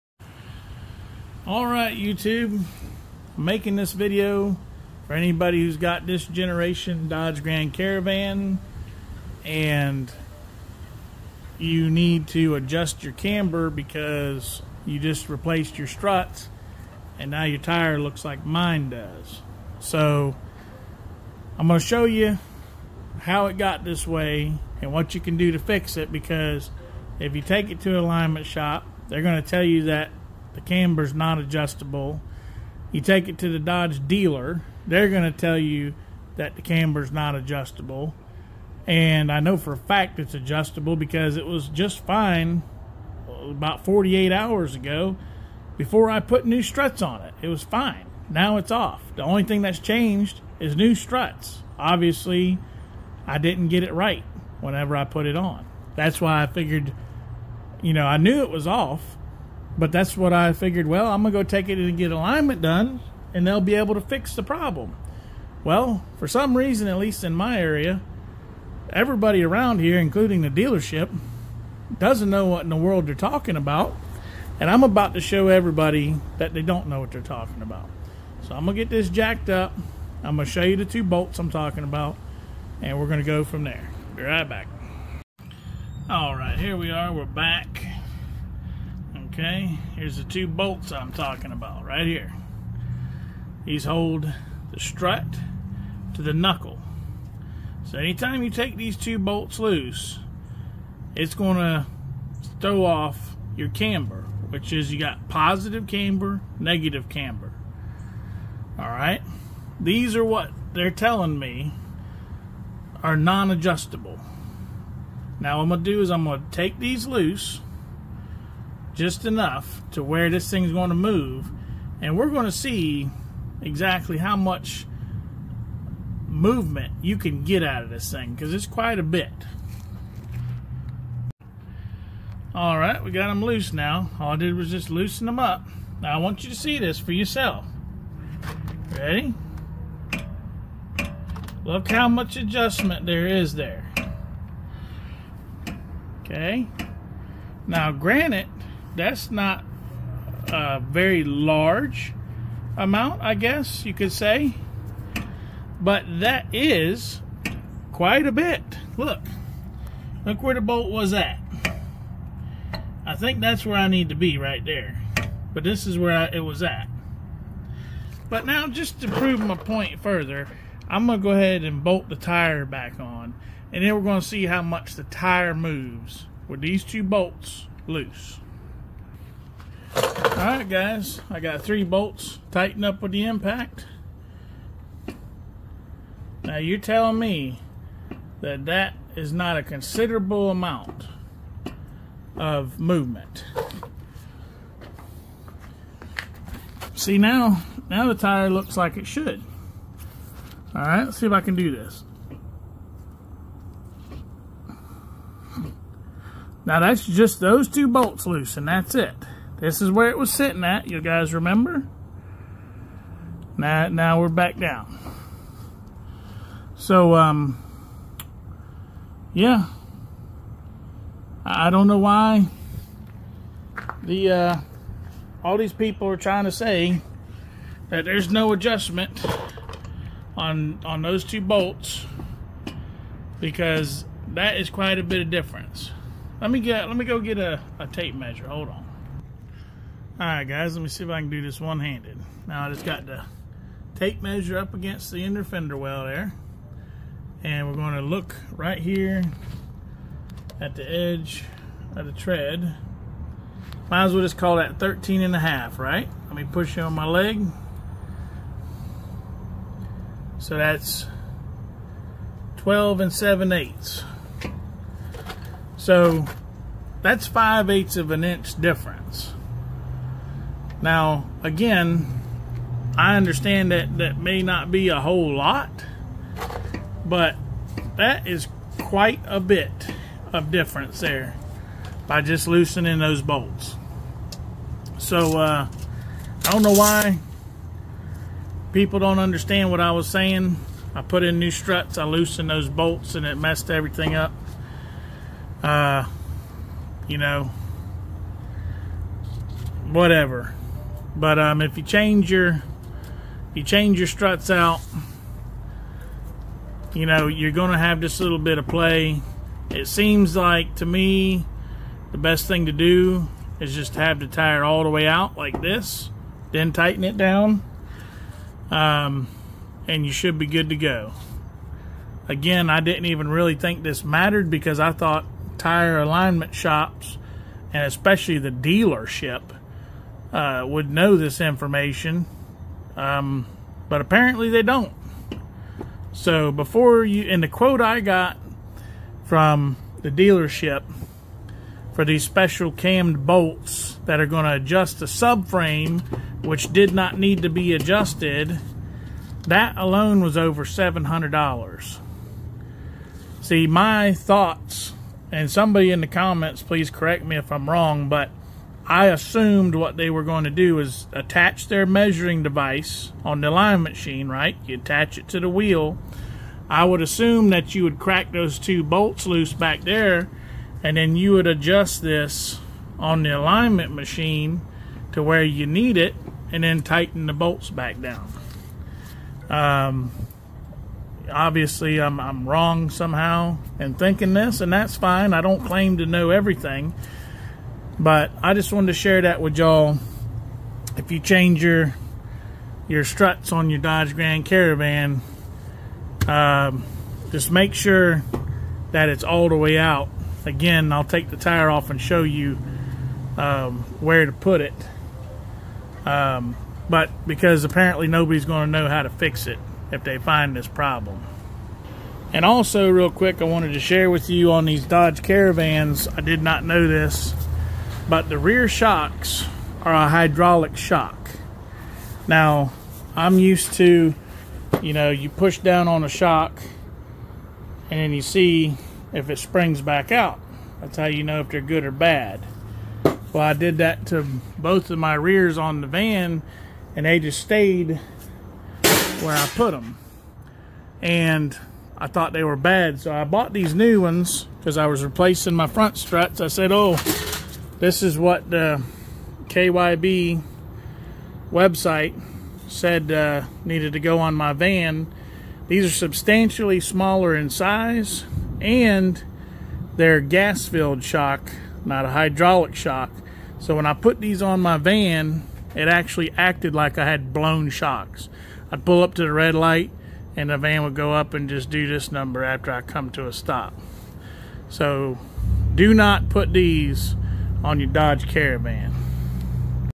5th Gen Dodge Grand Caravan Camber Adjustment
in this video I show how loosening the strut to knuckle bolts will change your alignment. Despite Tire Kingdom and my local Dodge dealership telling me that loosening these bolts would do nothing, I show you how much difference it makes.